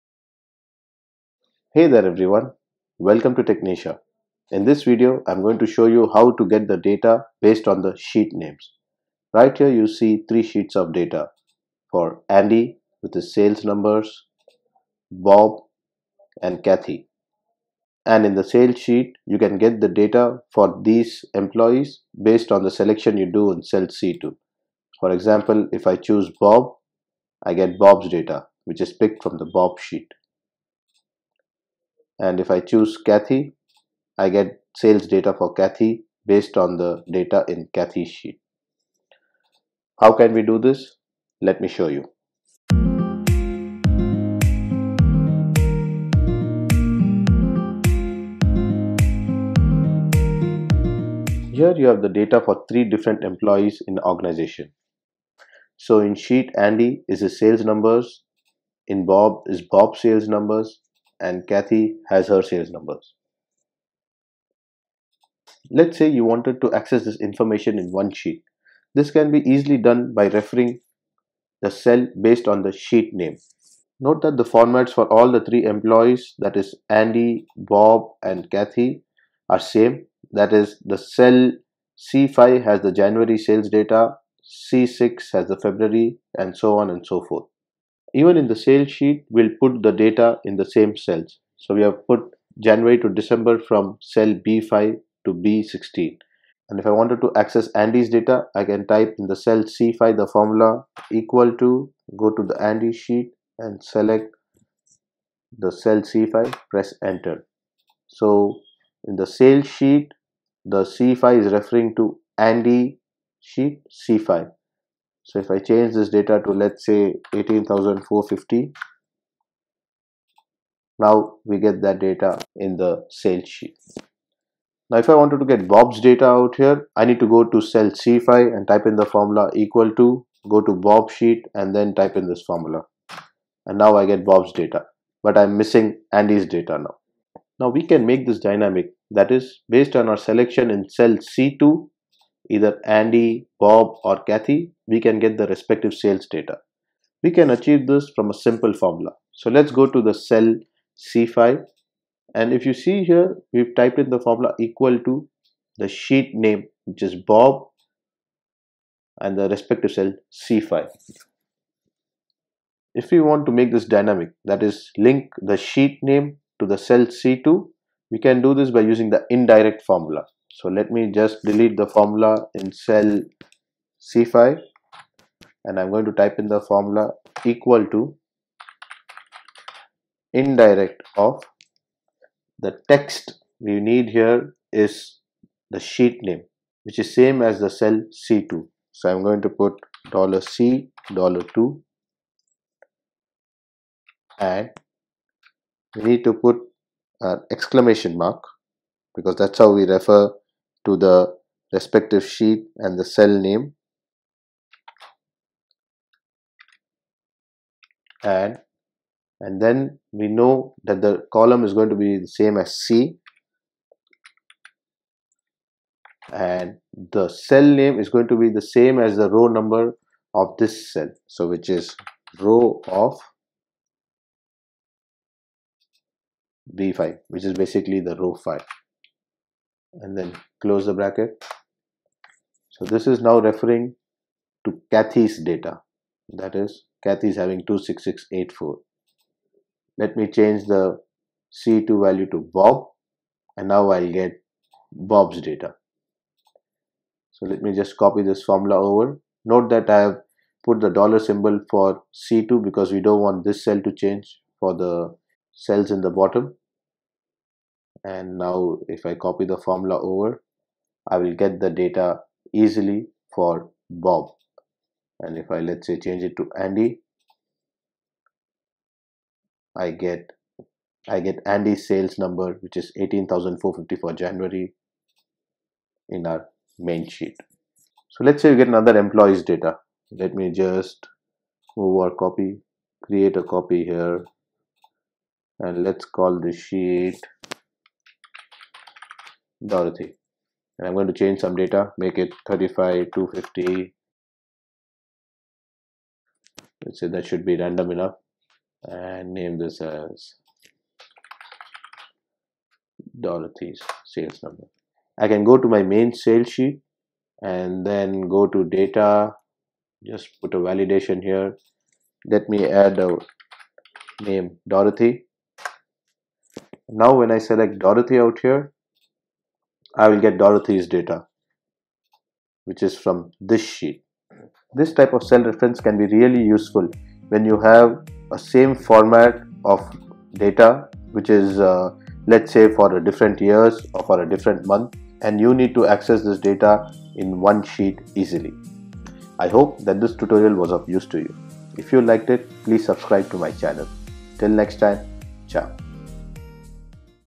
Excel Trick! Refer Data from different Sheets in the Same Cell
A simple and easy way to refer data from different sheets in the same cell